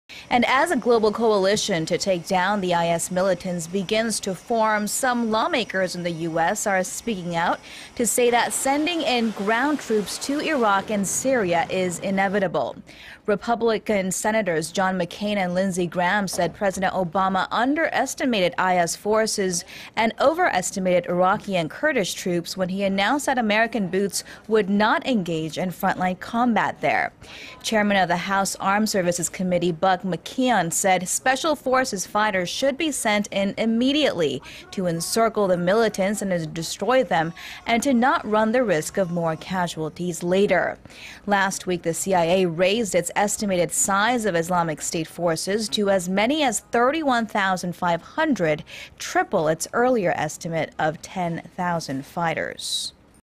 U.S. lawmakers say American ground troops will need to fight to prevail against
As a global coalition to take down the I-S militants begins to form... some lawmakers in the U.S. are speaking out to say that sending in ground troops to Iraq and Syria... is inevitable.
Republic senators John McCain and Lindsay Graham said President Obama underestimated I-S forces and overestimated Iraqi and Kurdish troops... when he announced that American boots would not engage in frontline combat there.
Chairman of the House Arms Services Committee Buck McKeon said Special Forces fighters should be sent in immediately to encircle the militants and destroy them... and to not run the risk of more casualties later.
Last week, the CIA raised its estimated size of Islamic State forces to as many as 31-thousand-500... triple its earlier estimate of 10-thousand fighters.